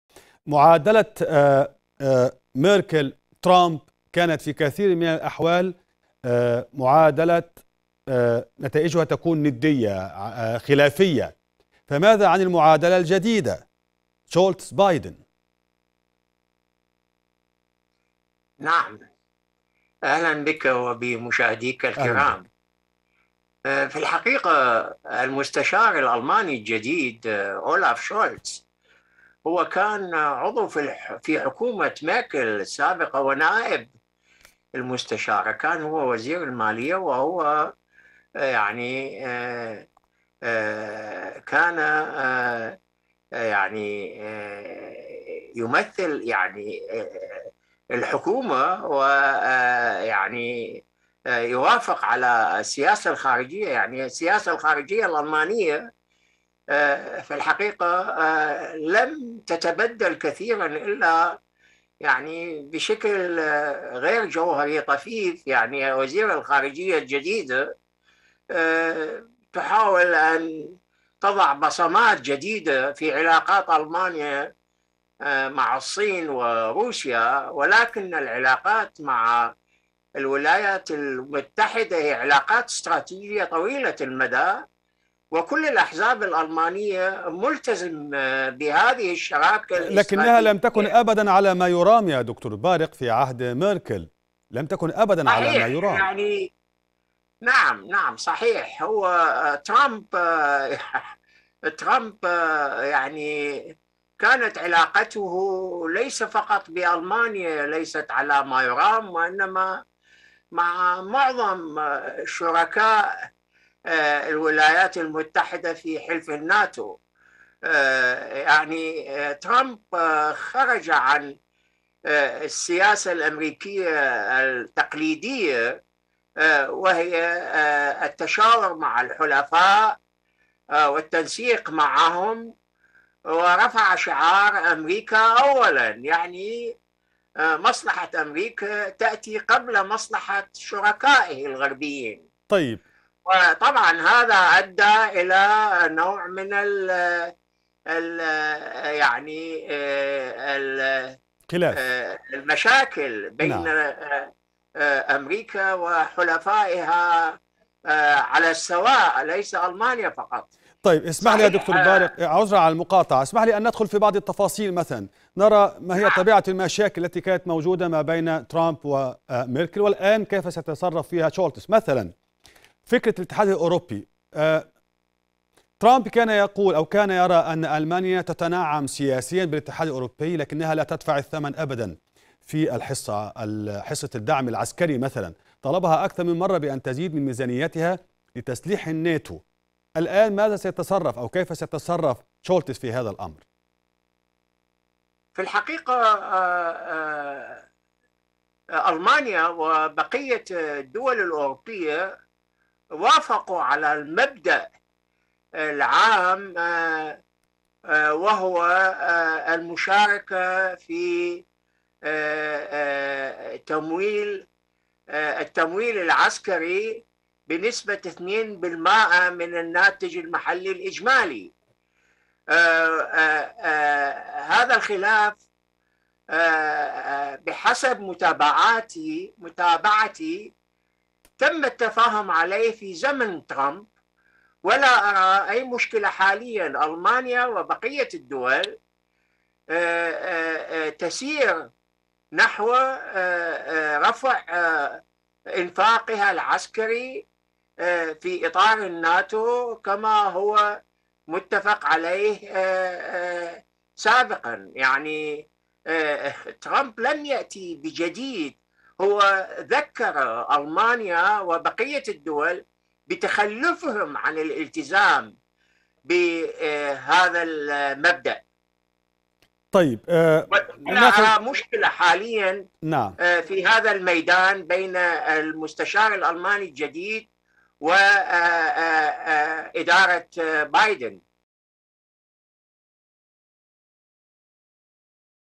معادلة «ميركل - ترامب».. كيف تختلف عن «شولتس - بايدن»؟
مواقف مشتبكة و مصالح مشتركة ، و بينهما تتشكل العلاقة_بين_برلين_وواشنطن .. فبعد ما أصاب تلك العلاقة من ضرر خلال عهد الرئيس الأمريكي السابق دونالد ترامب ، حاولت ميركل خلال ما تبقى من ولايتها الأخيرة ، إصلاح ذلك الضرر مع خليفته جو بايدن ، وسط توقعات بأن تتجه تلك العلاقة نحو الأفضل في عهد المستشار_الجديد_أولاف_شولتس ، والذي يرى مراقبون أن سياسة حزبه "الاشتراكي الديمقراطي" تشبه إلى حد كبير أيدولوجية الحزب الديمقراطي الأمريكي .
تفاصيل أكثر عن الموضوع مع ضيف قناة الغد من برلين، د. بارق شبر - الخبير في الشئون الألمانية والعلاقات الدولية